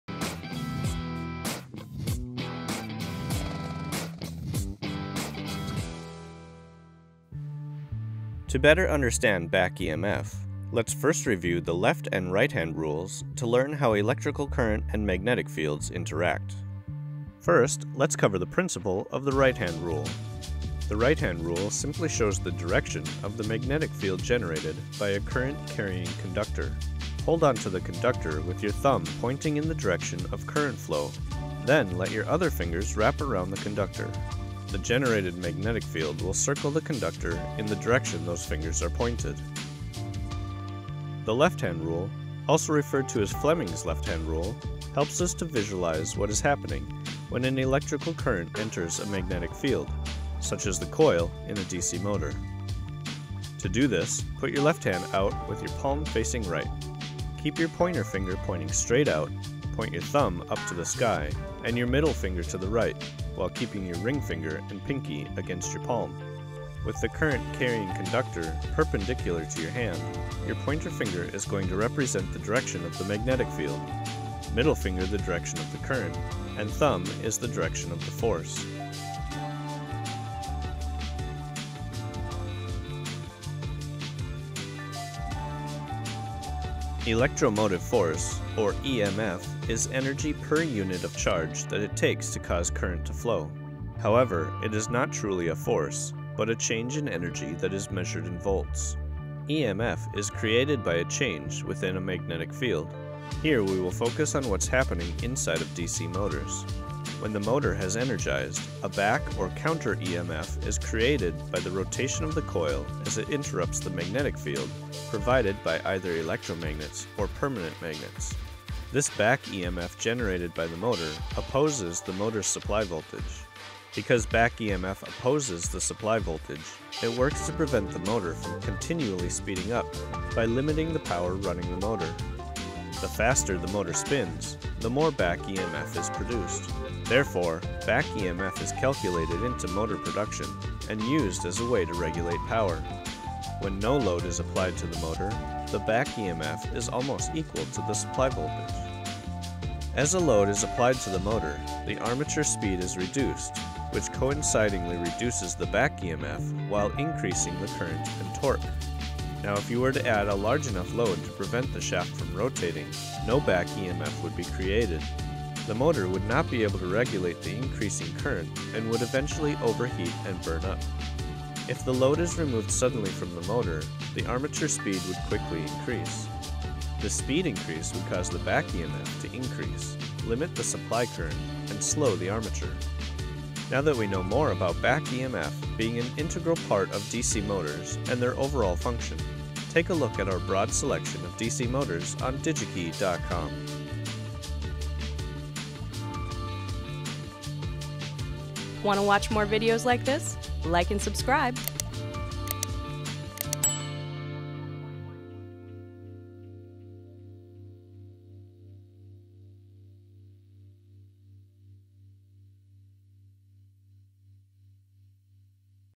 What is Back EMF in DC Motors? - Another Teaching Moment | Digi-Key Electronics
Applying power to a DC motor makes it rotate to do work but there’s much more going on inside the motor such as Back EMF. Back EMF generated by the motor’s rotating armature opposes the supply voltage. The faster the motor spins, the more back EMF is produced. Therefore, back EMF is calculated into motor design and efficiency. This video explains in greater detail how Back EMF effects DC motor supply power and varies depending upon applied loads.